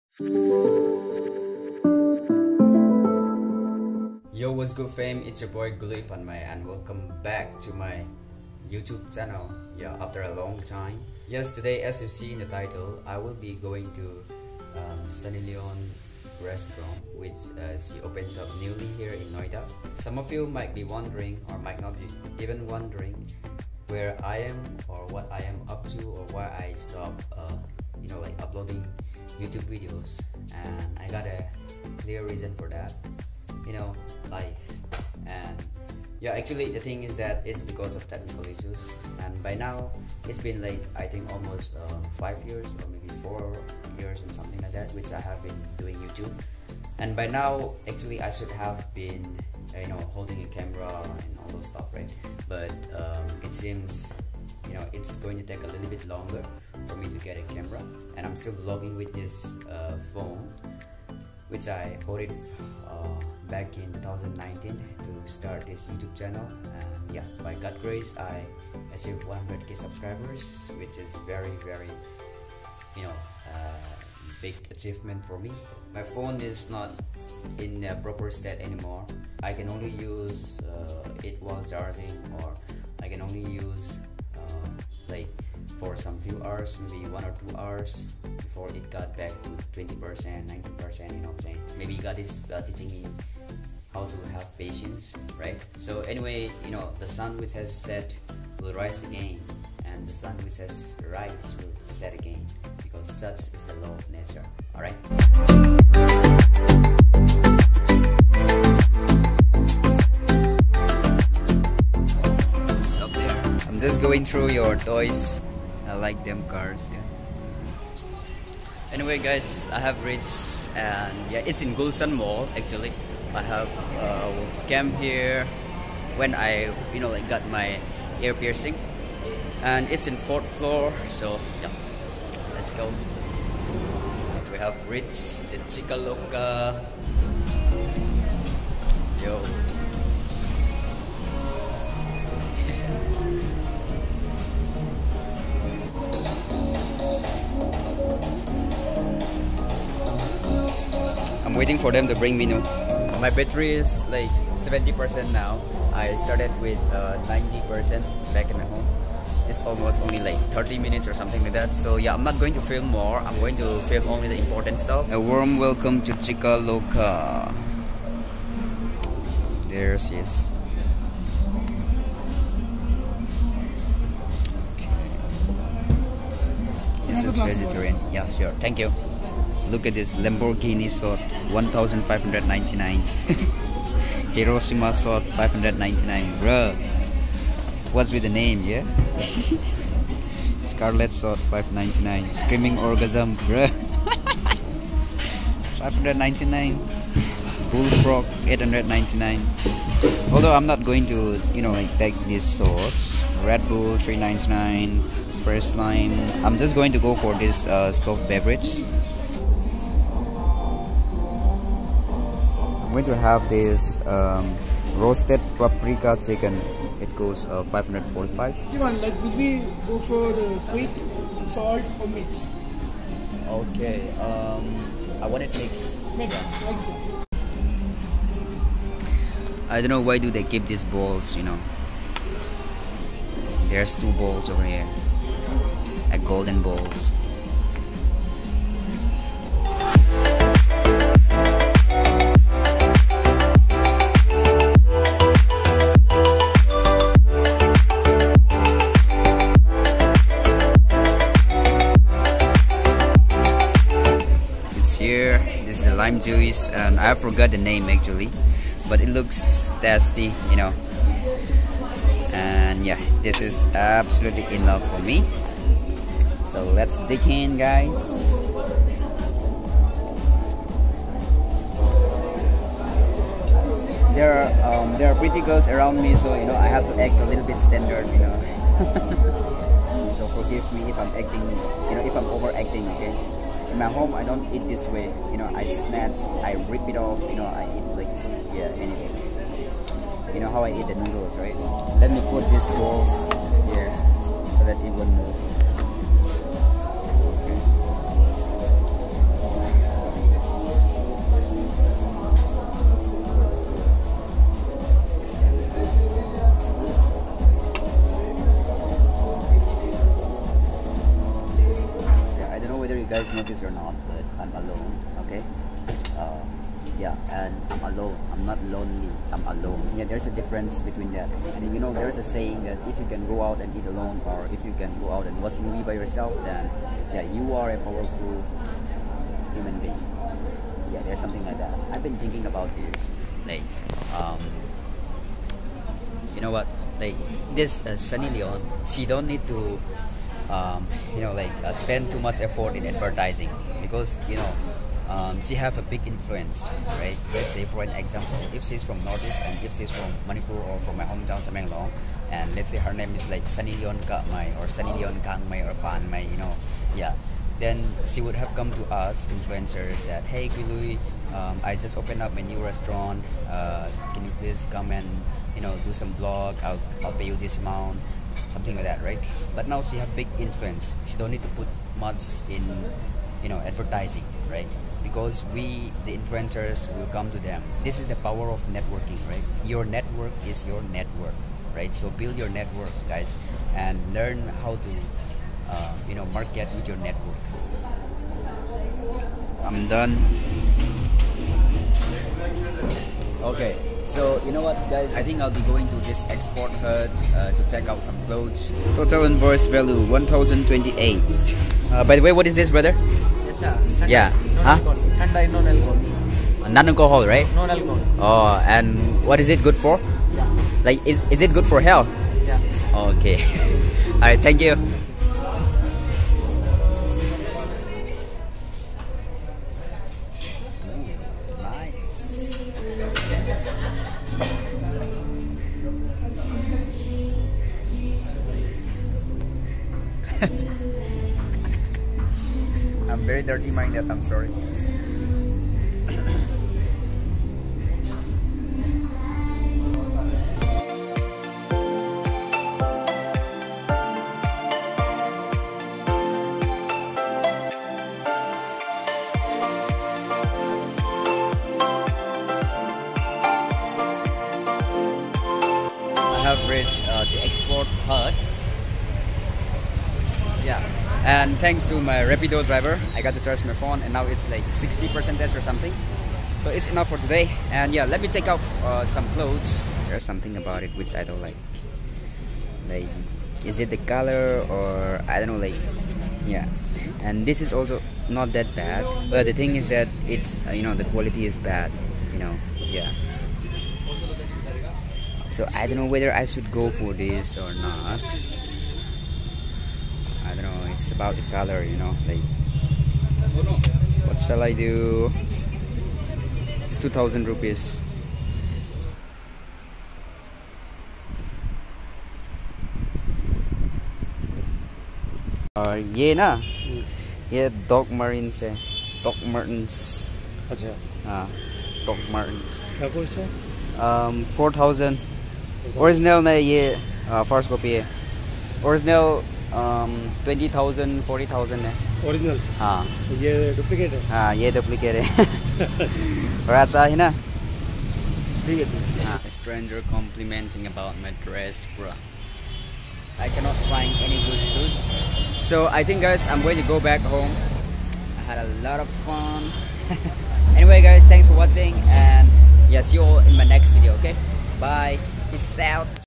I went to Sunny Leone Bar/Restaurant In Noida Delhi NCR || Guilui Panmei 2024
Location :- 4th Floor, Plot Number-C3, One29, E1, Noida-Greater Noida Expy, above Gulshan, Sector 129, Noida, Uttar Pradesh 201301

If you see this just know that you're beautiful 😍 and i love you 😘